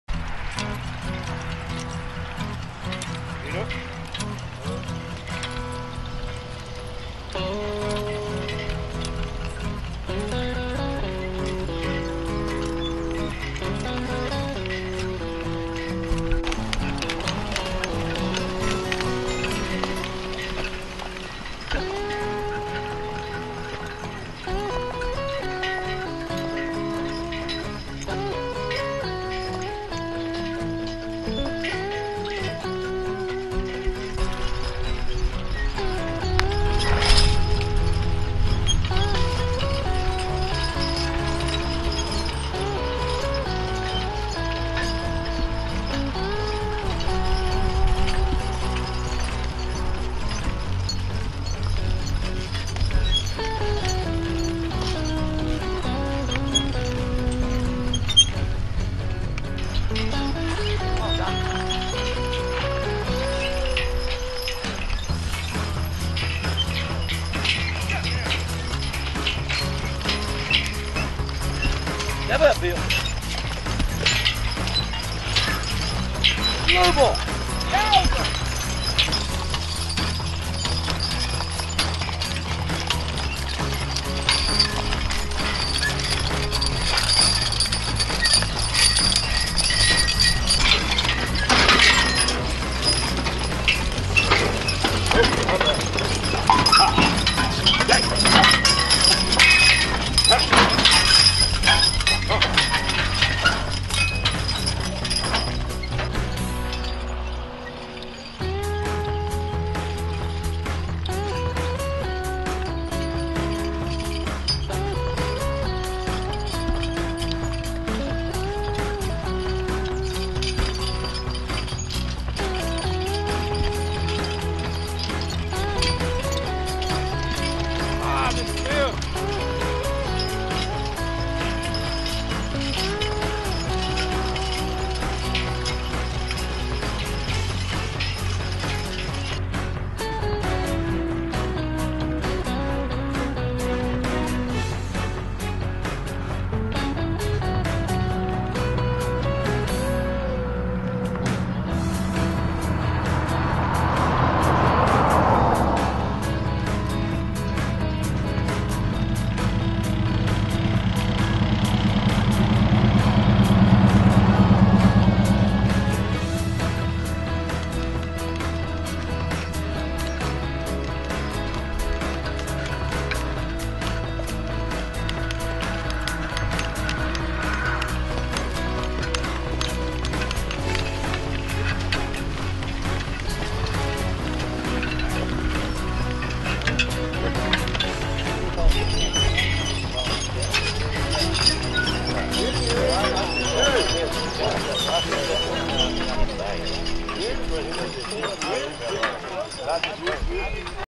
North Carolina Work Horse and Mule Association Goldsboro Plow Day
If you enjoy this video please check the LIKE box and share with others. Thanks for visiting.
Goldsboro Plow day was held on the Jeff Walker farm in Goldsboro, North Carolina on November 9th, 2013. A perfect day for plowing was enjoyed by visitors and teamsters alike Two  teams from Salisbury, Maryland participated in the plowing.